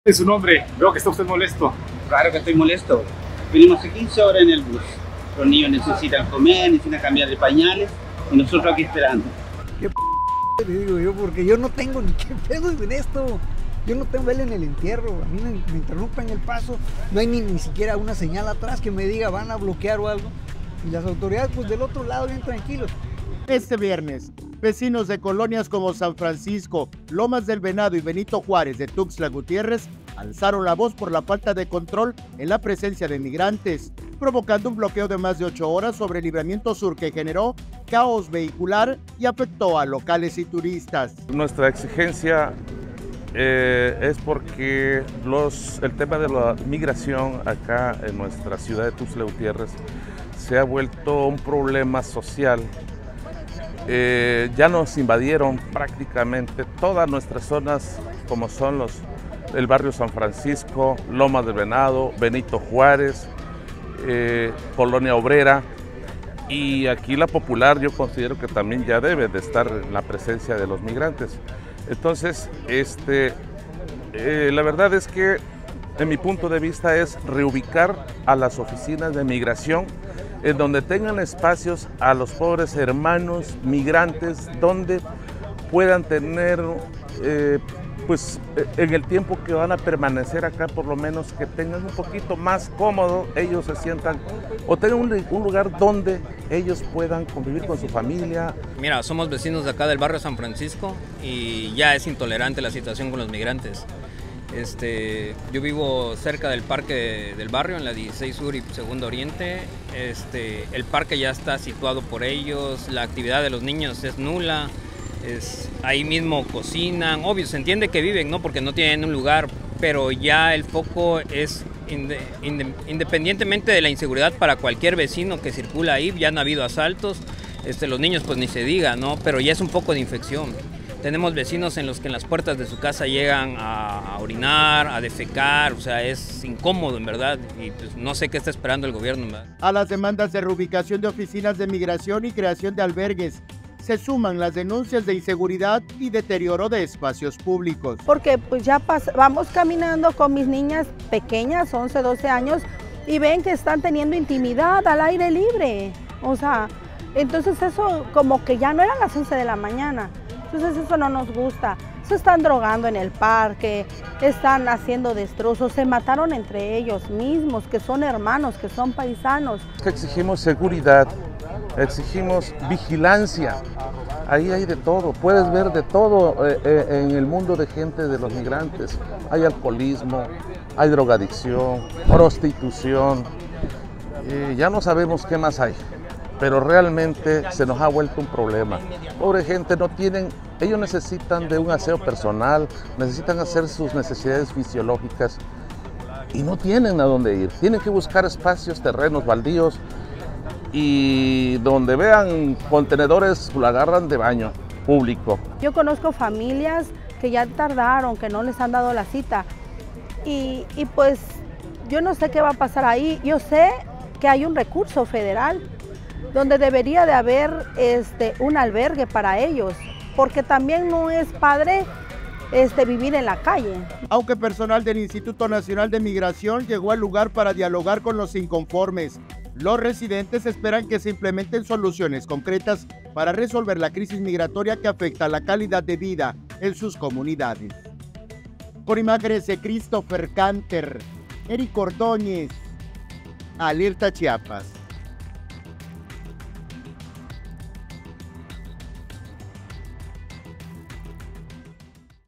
¡Bloqueo desquicia inicio de fin de semana en Tuxtla!
Habitantes de barrios como San Francisco y Lomas del Venado denuncian inseguridad, insalubridad y saturación de espacios públicos.